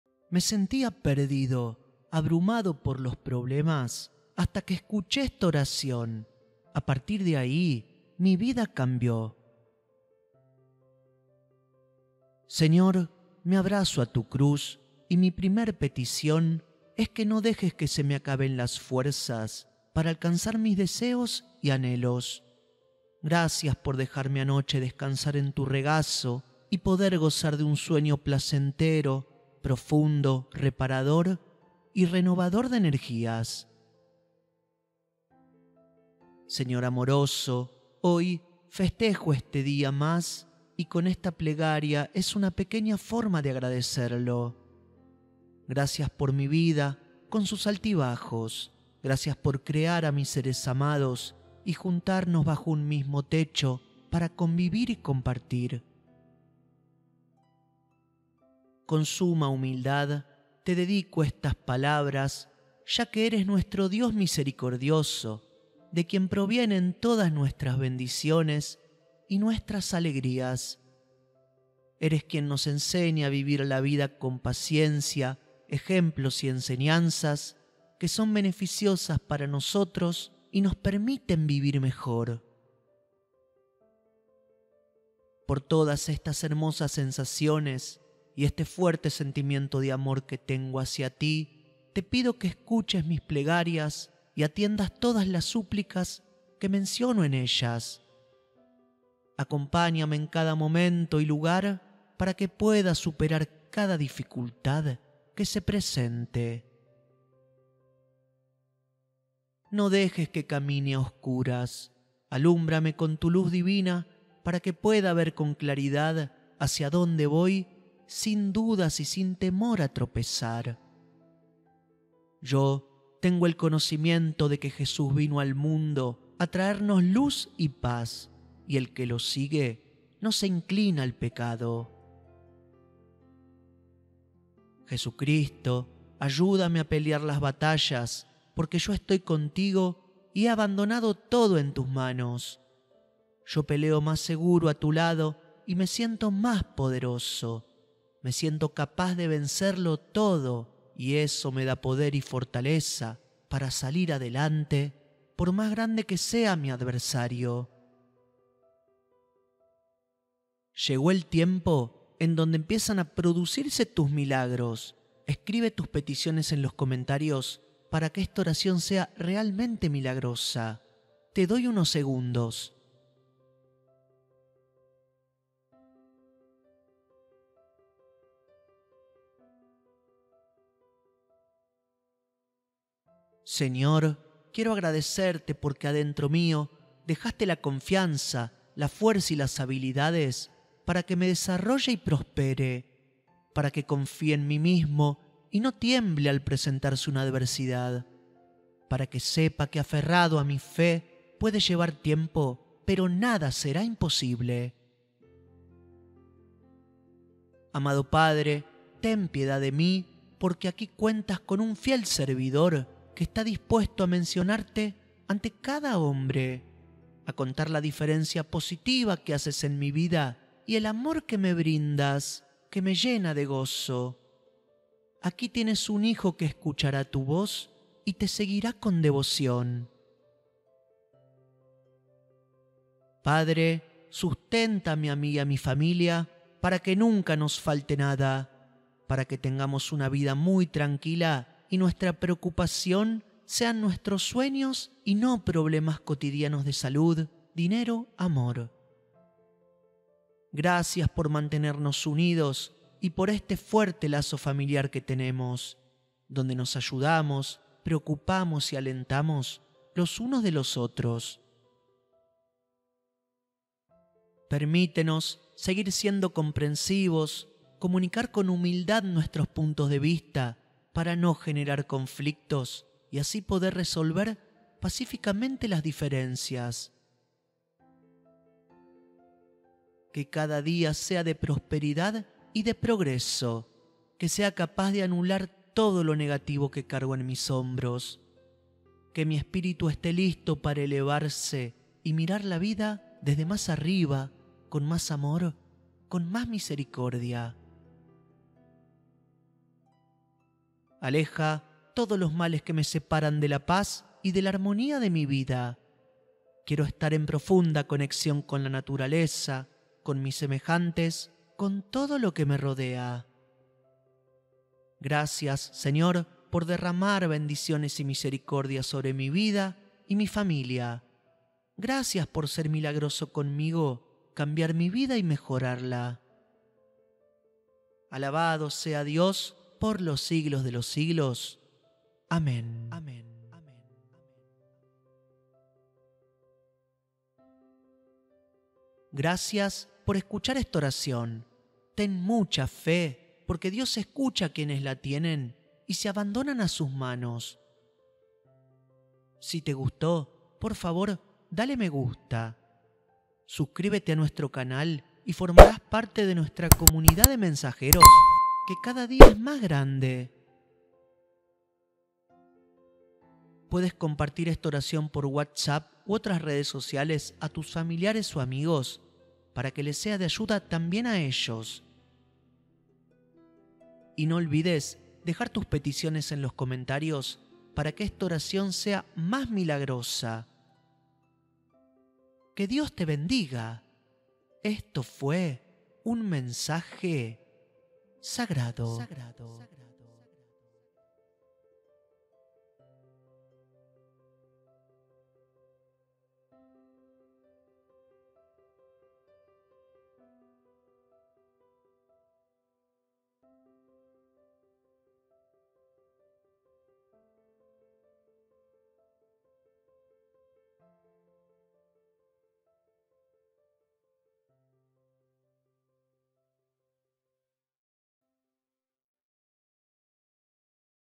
😇Tuve fe en el Señor y al fin logré lo que tanto soñaba😇
Señor amoroso, hoy festejo este día más, y con esta plegaria es una pequeña forma de agradecerlo.
Gracias por mi vida con sus altibajos, gracias por crear a mis seres amados y juntarnos bajo un mismo techo para convivir y compartir.